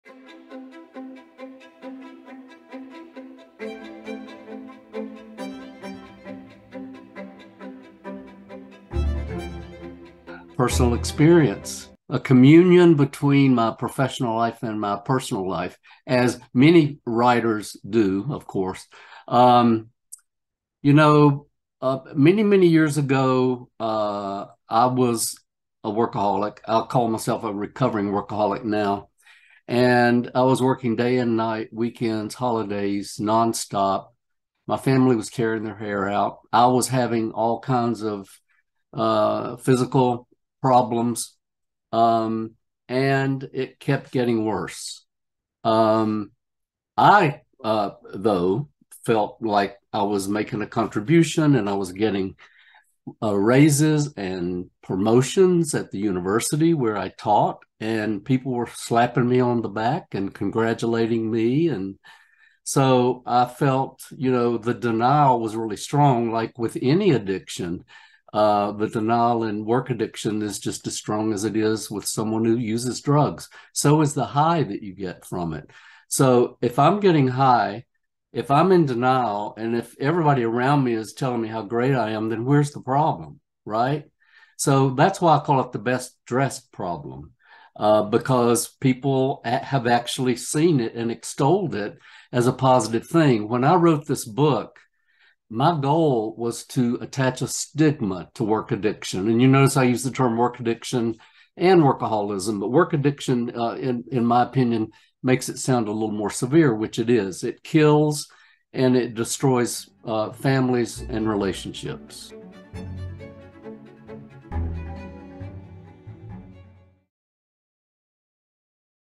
Chained to the Desk in a Hybrid World - Bryan Robinson
Bryan Robinson discusses the impact of work stress on those who live and work with workaholics—partners, spouses, children, and colleagues—as well as the appropriate techniques for clinicians who treat them.